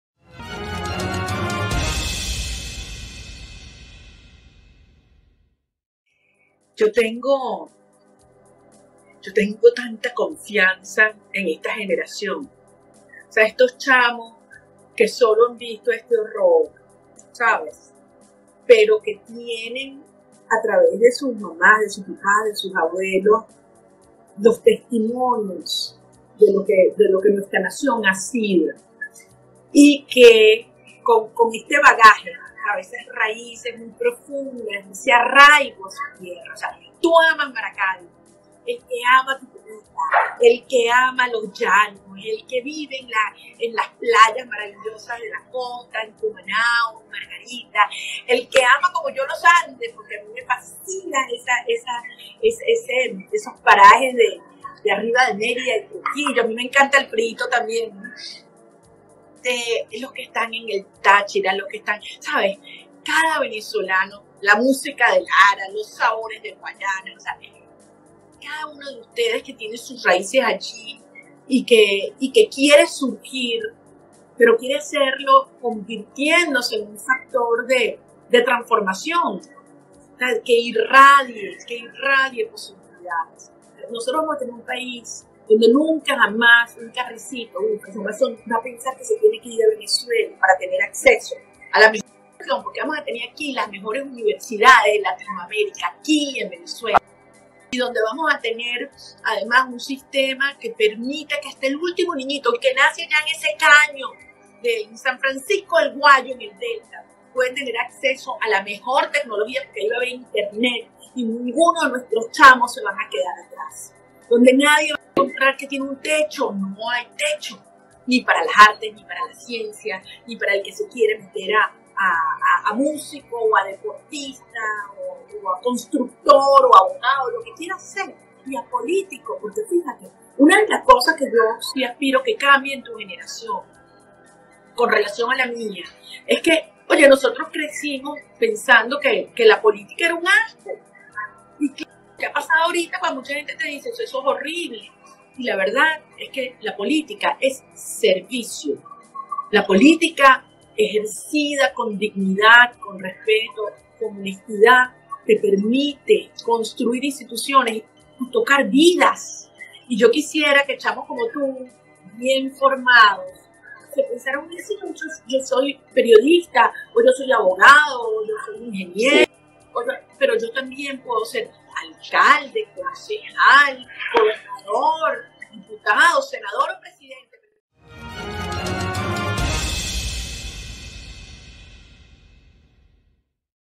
EMOTIVO MENSAJE MARIA CORINA MACHADO A LOS JÓVENES VENEZOLANOS
Maria Corina Machado a los Jóvenes Venezolanos